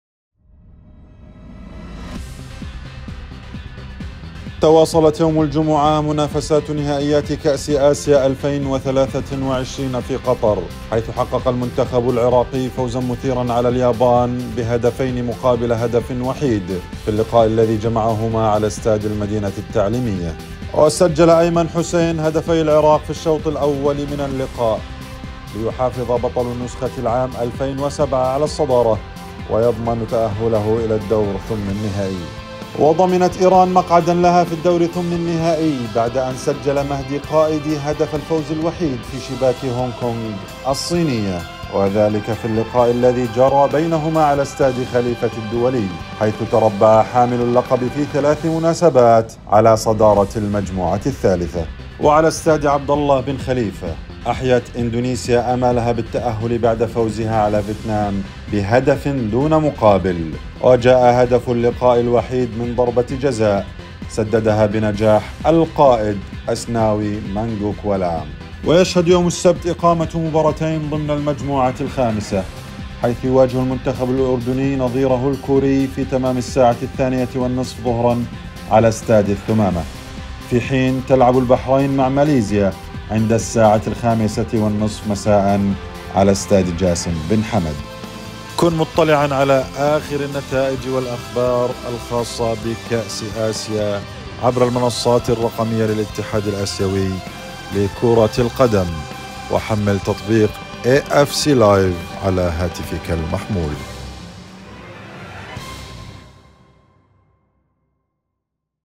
AsianCup2023: التقرير اليومي: 20 يناير
بدأت يوم الجمعة في نهائيات كأس آسيا 2023 في قطر بمفاجأة، حيث حقق العراق فوزاً مثيراً على اليابان على ستاد المدينة التعليمية. سجل أيمن حسين هدفين في الشوط الأول لبطل 2007، وعلى الرغم من تلقي شباكه هدفين متأخرين، حافظ العراق على صدارة المجموعة الرابعة وتأهل إلى دور الـ16.